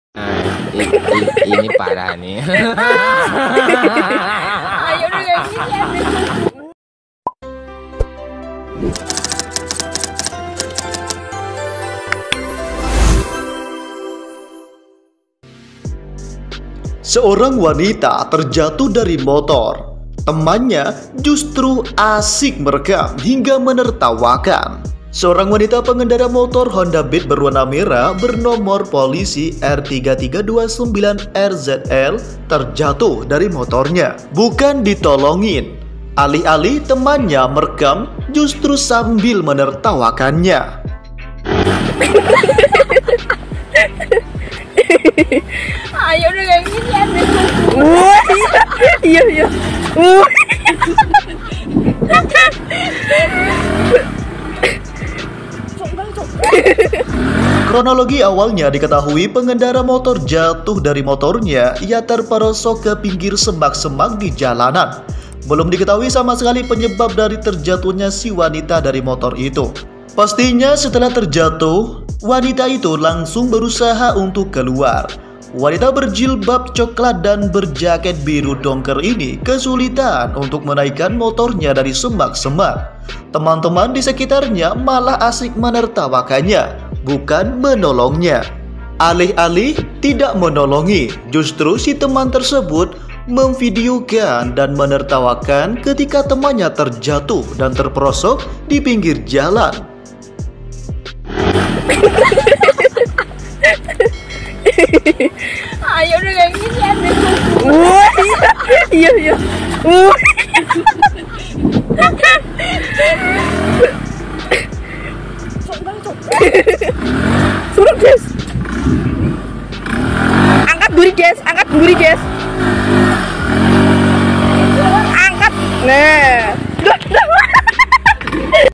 TEMAN BIADAB ‼️WANITA INI JATUH DARI MOTOR MALAH DI REKAM
Berikut berita yang sedang viral saat ini mengenai Seorang Wanita yang terjatuh dari motor tapi temannya malah merekam hingga menertawakan.

Seorang Wanita pengendara motor Honda Beat berwarna merah bernomor polisi (nopol) R 3329 RZL terlibat dalam kejadian ini. Kedua teman wanita lainnya juga terlibat dalam kejadian ini.

Wanita yang mengendarai motor Honda Beat ini tiba-tiba terjatuh. Kedua temannya malah asyik merekam dan menertawakan Wanita itu

Melihat nopolnya kejadian berlangsung di sekitar Banjarnegara, Banyumas, Cilacap dan Purbalingga. Tidak diketahui apakah memang pengendara motor sedang persis berada di salah satu daerah itu.